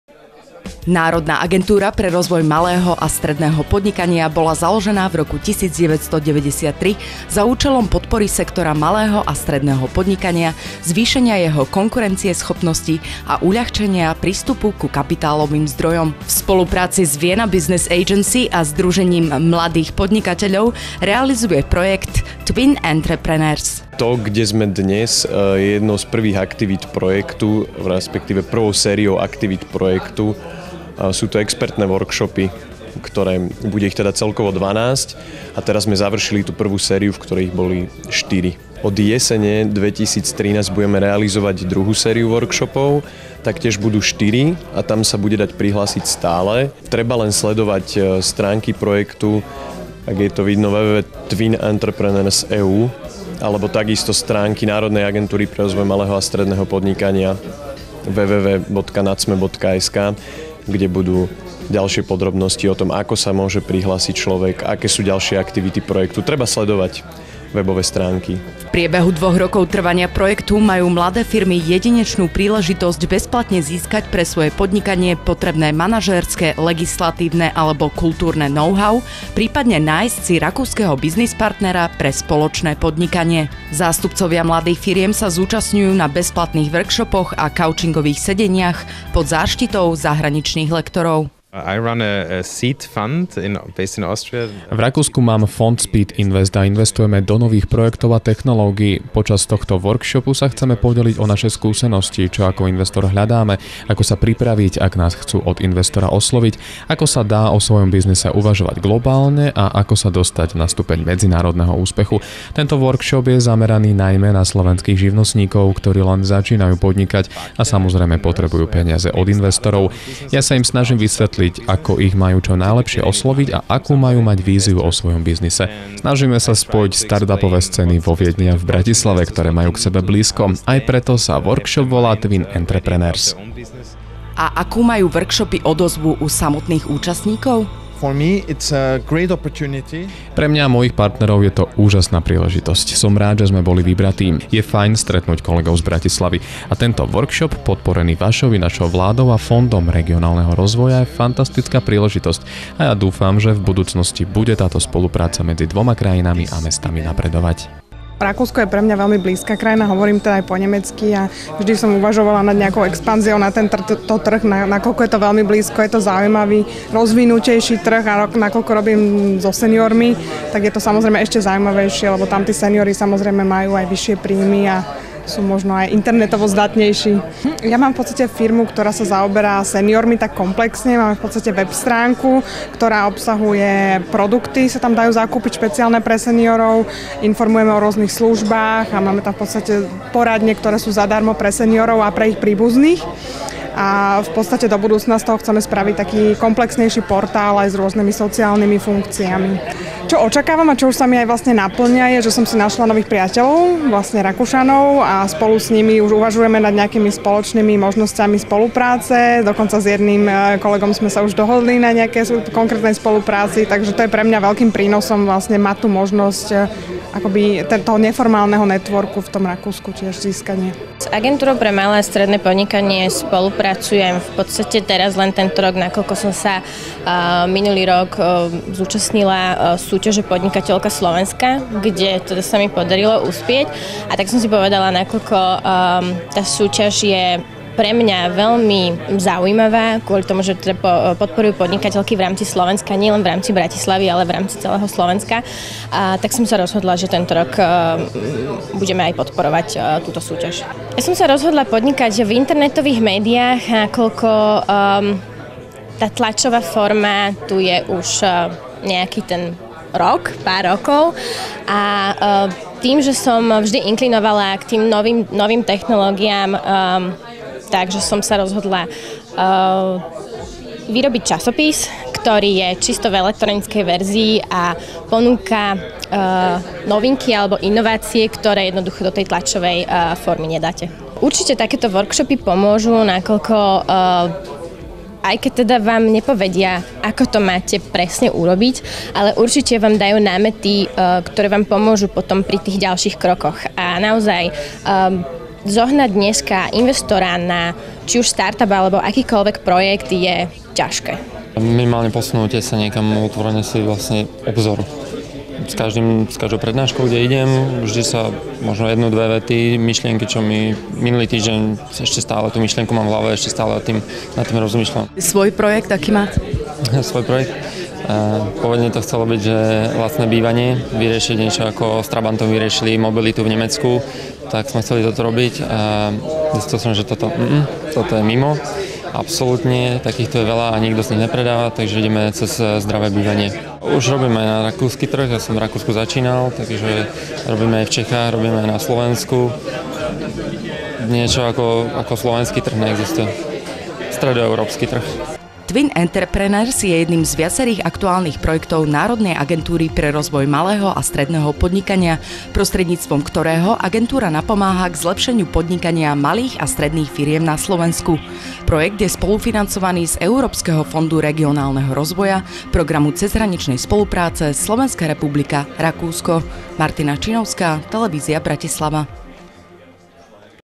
Workshop k projektu TwinEntrepreneurs
Televízna reportáž (TV Bratislava) z workshopu k projektu TwinEntrepreneurs, ktorého podstatou je spolupráca podnikateľov z regiónu Viedne a Bratislavy. TwinEntrepreneurs realizuje Národná agentúra pre rozvoj malého a stredného podnikania v spolupráci s Vienna Business Agency a Združením mladých podnikateľov.  Ešte stále je možné sa prihlásiť do ďalších workshopov, ktoré začnú v jeseni. Viac informácií nájdete na stránke